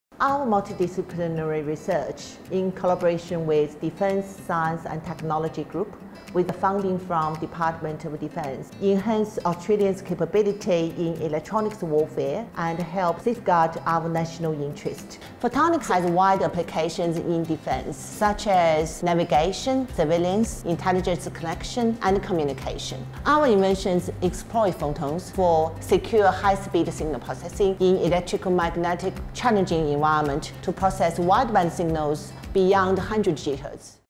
Photonic Radio Frequency@Sydney, finalist 2016 Eureka Prize for Science in Safeguarding Australia
2016 Defence Science and Technology Eureka Prize for Outstanding Science in Safeguarding Australia
Photonic Radio Frequency@Sydney, University of Sydney
The research of Associate Professor Xiaoke Yi and Dr Linh Nguyen leverages breakthroughs in miniaturised photonic integrated circuit approaches, building a high sensitivity, ultra-wideband and tuneable radio frequency (RF) front-end system. The system solves technical challenges in defence platforms where size, weight and power are key issues.
The views expressed in these films are those of the finalists and do not necessarily reflect those of the Australian Museum.